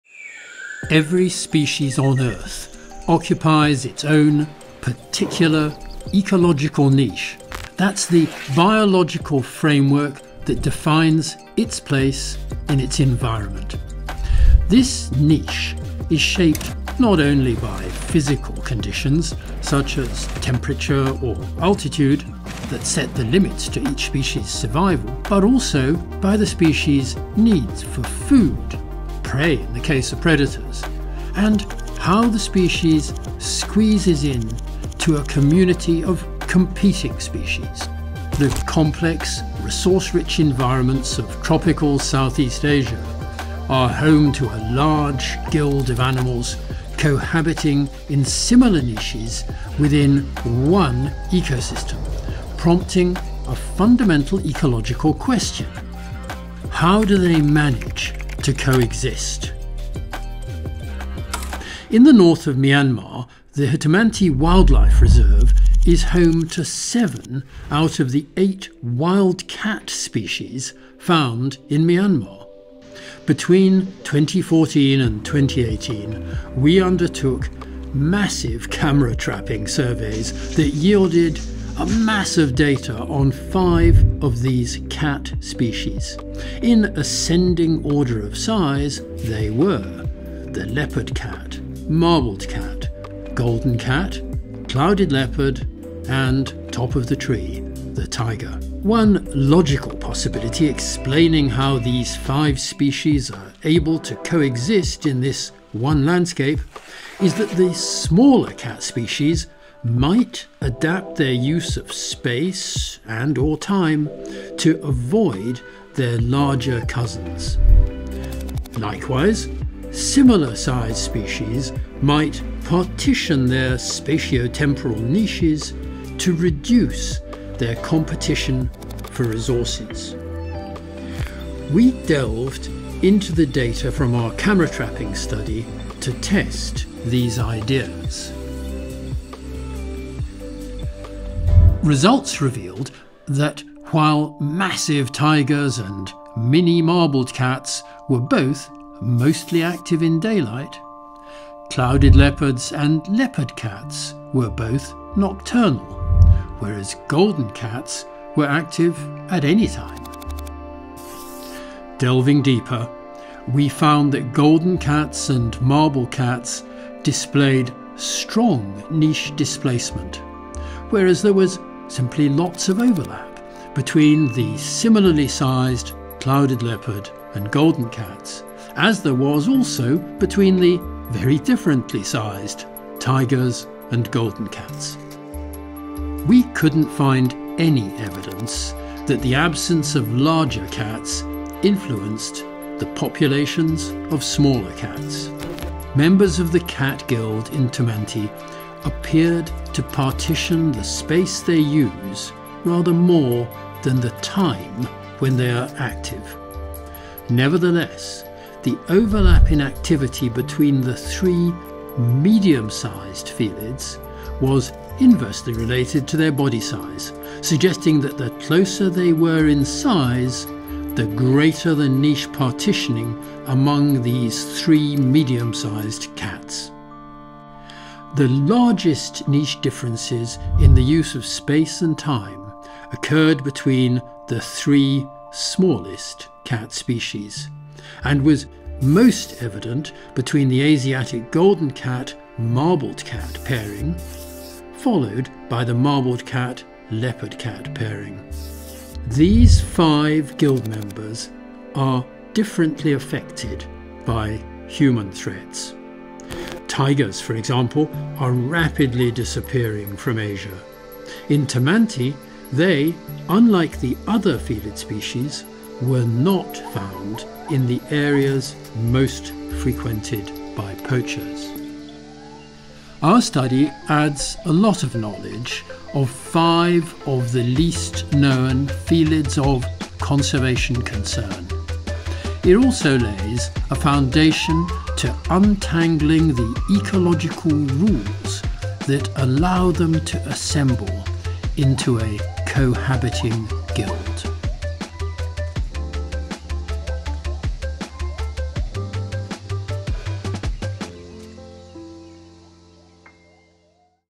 How Cats Share a Forest
Every species on earth occupies its own particular ‘ecological niche’: the biological framework defining its place in its environment. This niche is shaped not only by the physical conditions – such as temperature or altitude – that set the limits to its survival, but also by its need for food – prey in the case of predators - and how it squeezes into a community of competitors.

The complex, resource rich environments of tropical South East Asia, are home to large guilds of animals cohabiting in similar niches within one ecosystem, prompting a fundamental ecological question: how do they manage to coexist?